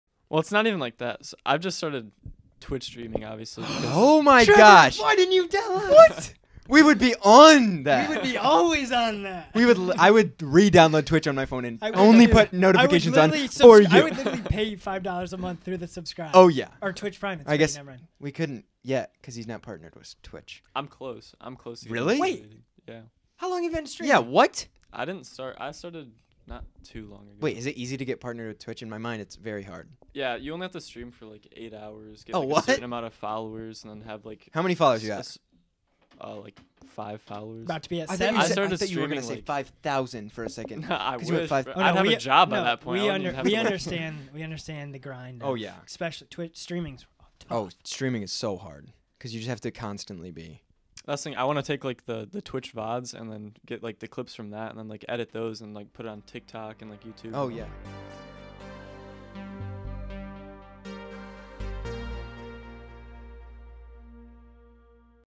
Trevor Olson Announces his new Twitch Account!
Main Channel: @Sukavision

Trevor just joined the Twitch streaming scene, and we had no idea about it until this podcast. Now we have been on his stream a few times! If you want a fantastic source of entertainment, make sure to go check out Trevor's stream in the link above.